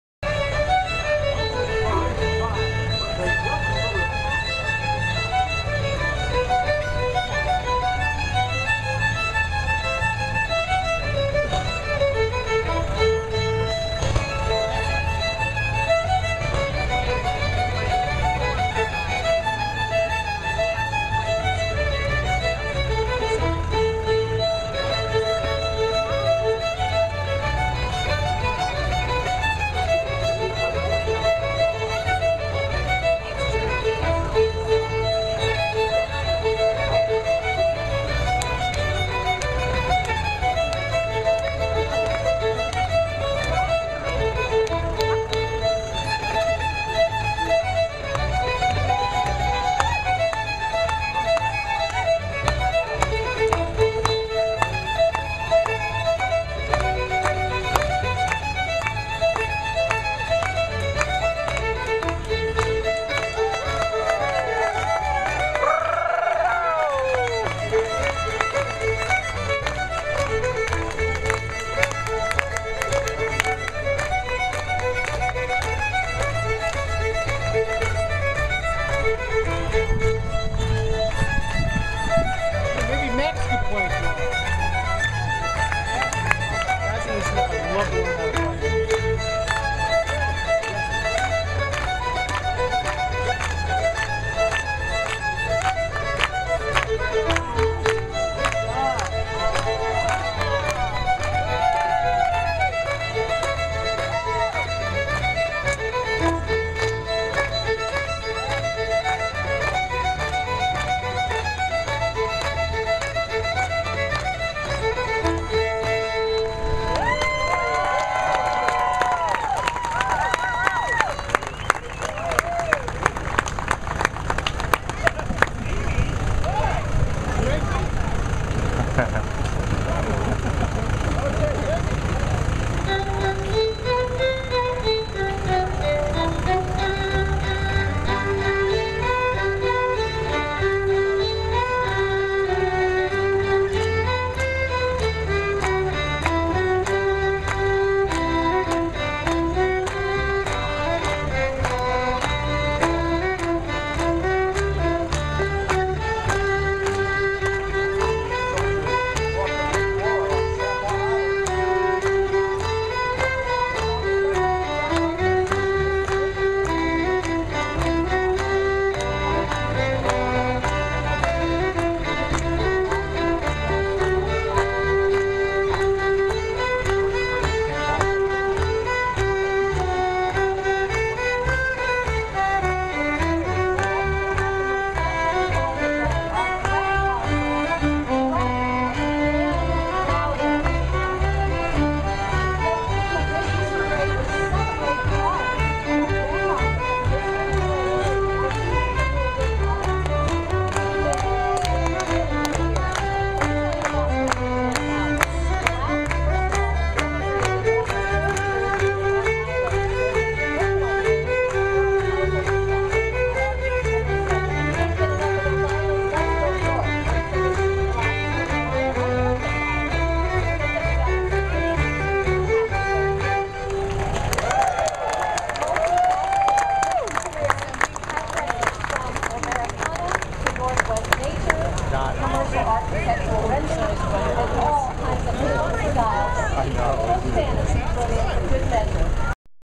Islay Faie Playing Violin in Fourth of July Parade on Orcas
Here is Islay playing her violin in the Fourth of July parade on Orcas Island.